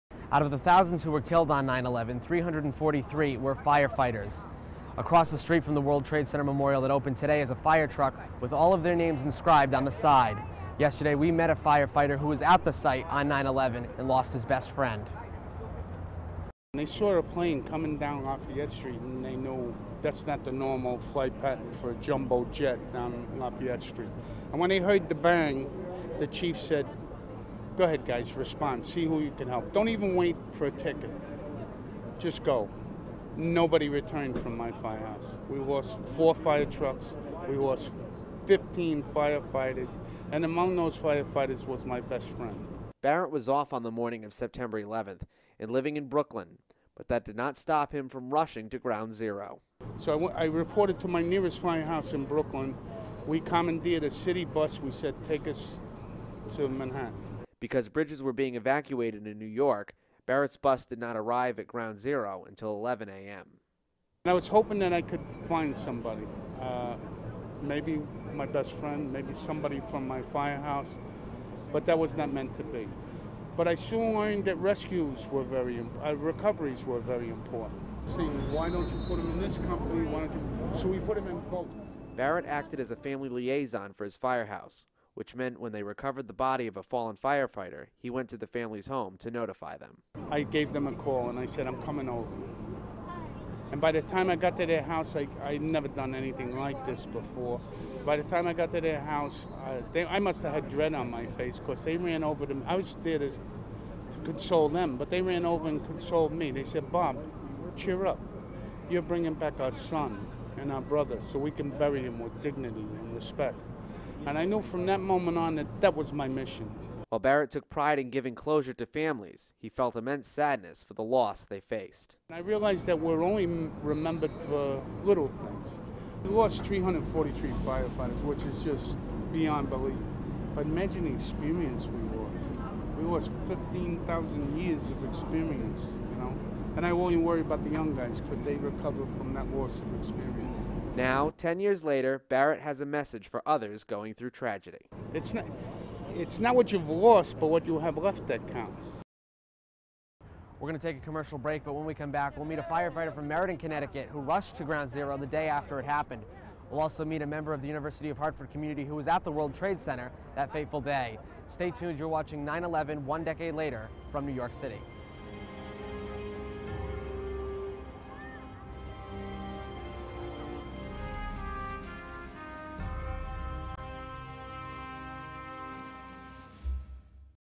FDNY fire fighter remembers 9/11 ten years later
Ben Wilcox interviews a New York Fire Fighter who was at ground zero on 9/11/01 and lost every member of his fire house. The interview took place at the world trade center on 9/10/11